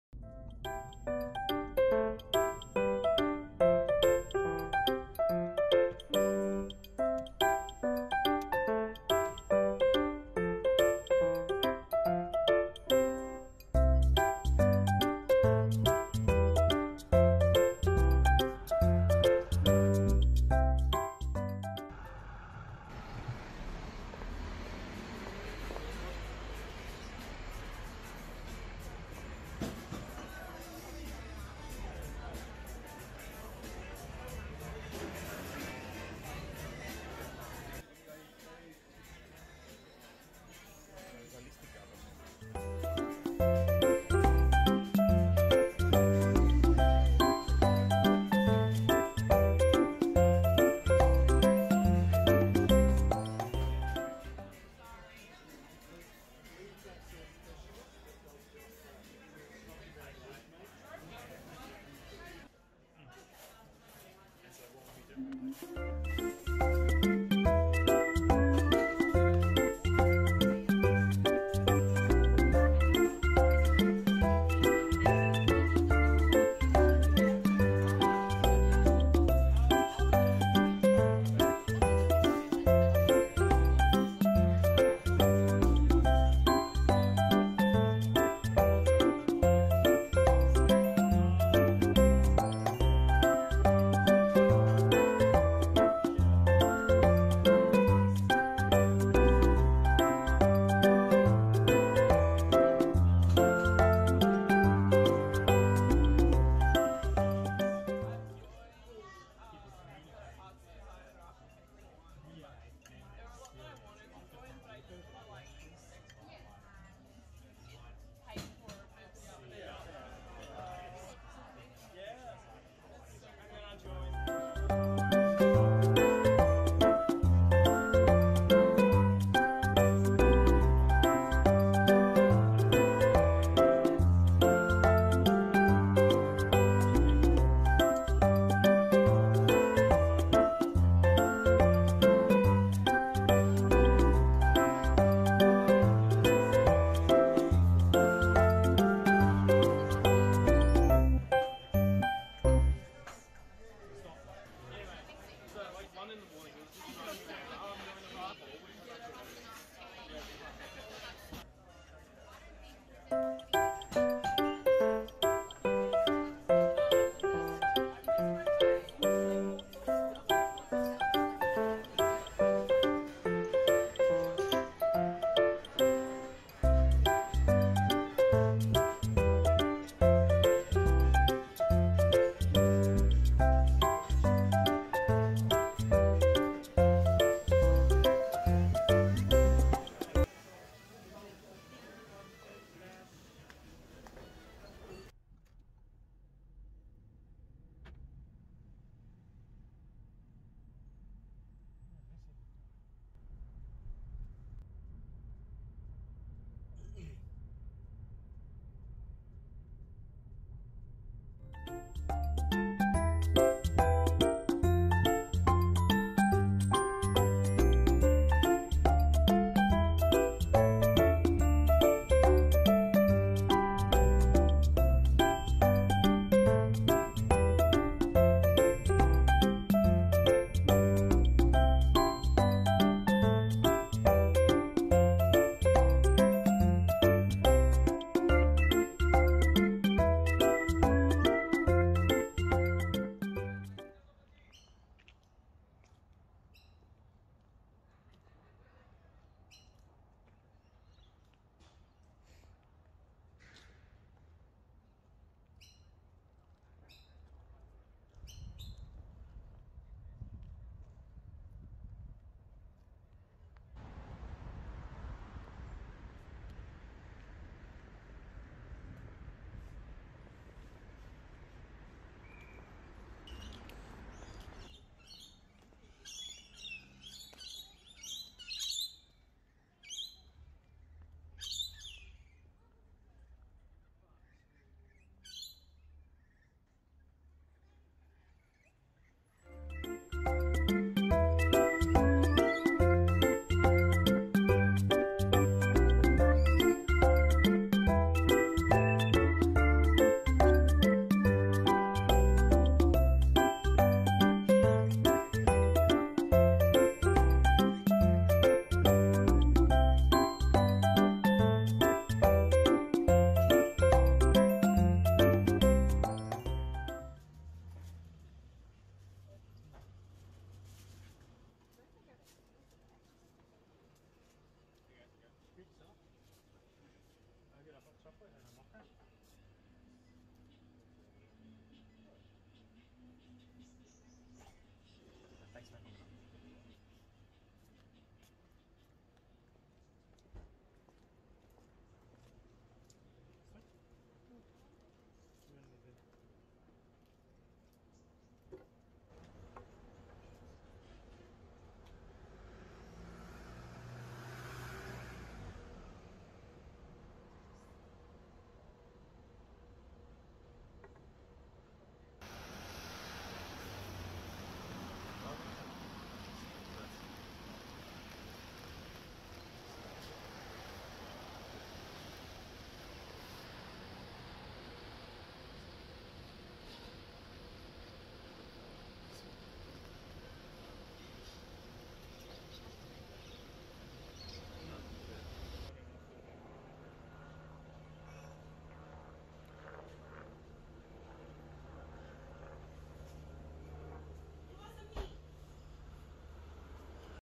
멜번 브이로그 | Melbourne vlog | Ned's Bake | Fawkner Park | Entrecote | 멜번 카페 | 부부 일상
토요일 일상 브이로그에요.
저희 부부의 단골 브런치 카페와 자주가는 공원에서의 일상을 담았어요.

Saturday vlog.
One of our favourite brunch cafes and parks in this vlog.